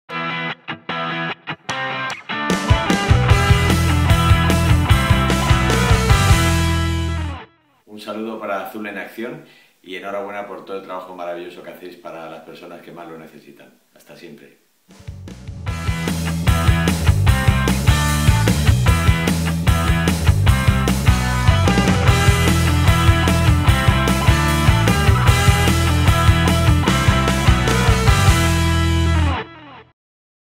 Antonio Hidalgo para Azul en Acción
El famoso presentador de Tv, Antonio Hidalgo, nos manda su saludo y apoyo a Azul en Acción.

Faebook: Azul en Acción Ong